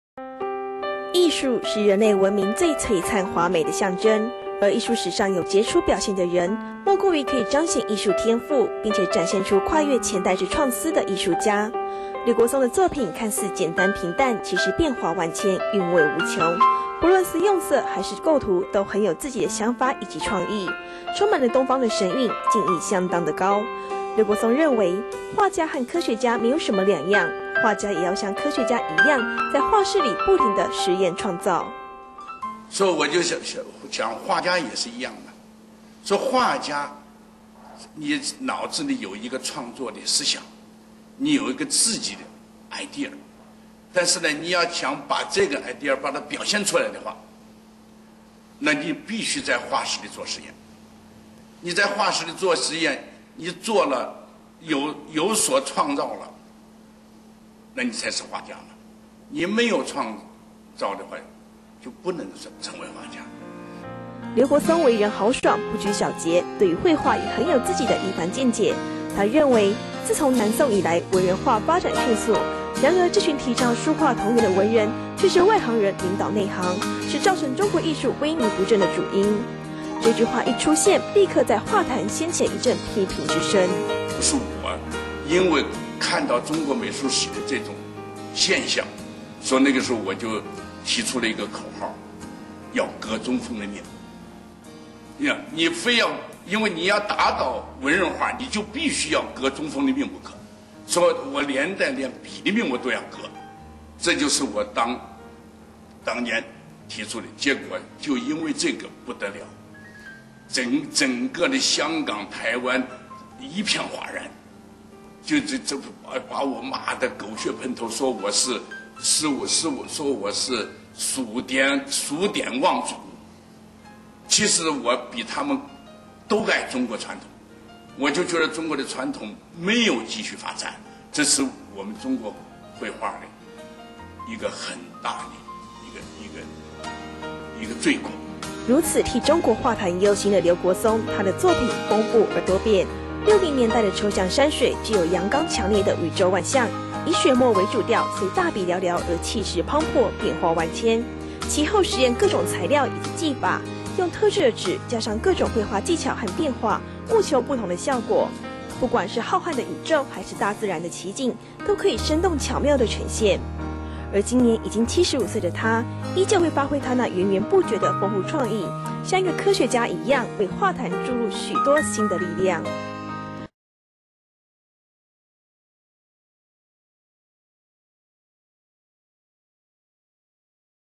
台灣現代水墨畫之父－劉國松
1932年出生的劉國松， 1957年與校友組成『五月畫會』，至今影響台灣現代藝術甚鉅。從早年完整嚴格的學院派訓練，繼而發展出獨創藝術風貌，最後揚名中外，奠定畫壇大師之地位。這段顯耀風光的歷程，對劉國松先生則只是衷心地追尋自我顯現的過程而已。